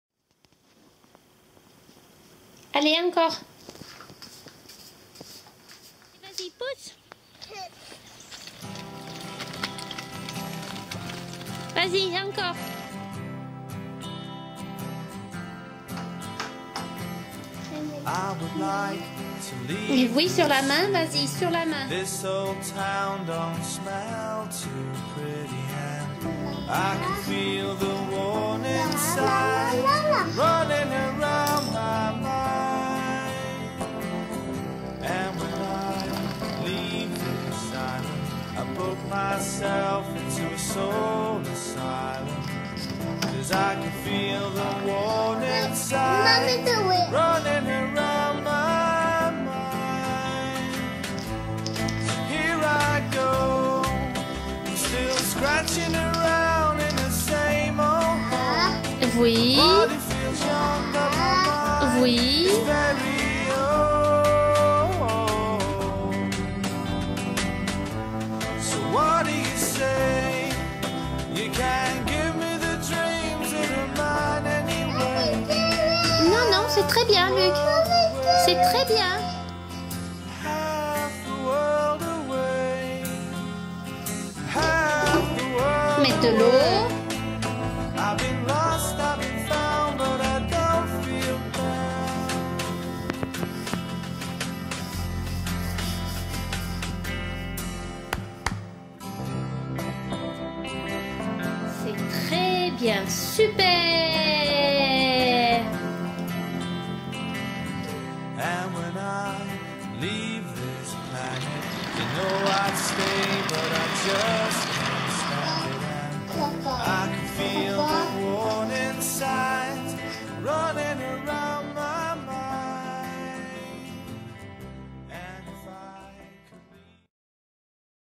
luke a world away
Luke painting, scooting, biking,  in and around home.
December 2016.
Aged 3 years and a bit.